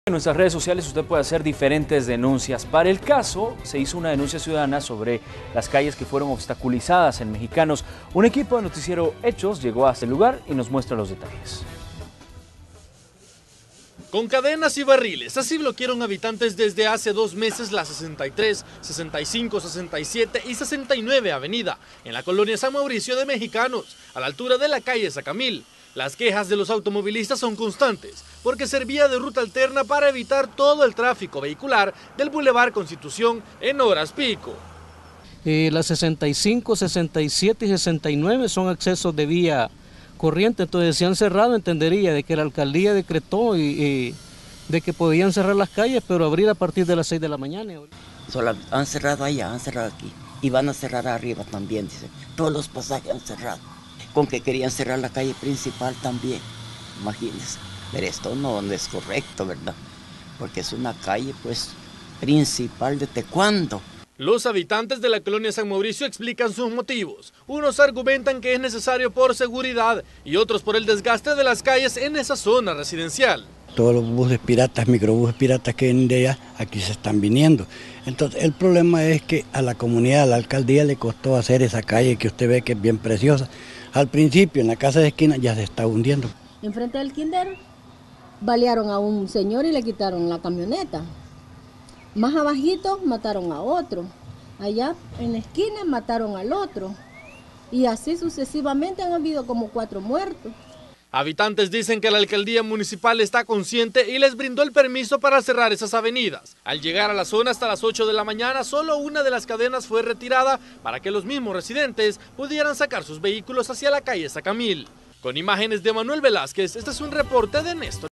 Habitantes cierran avenidas en Mejicanos
La denuncia ciudadana en este caso, alertó de que los habitantes cierran avenidas en Mejicanos para evitar que circulen vehículos. Las quejas de los automovilistas son constantes.  Dicen que tienen permiso de la alcaldía para cerrar estas avenidas.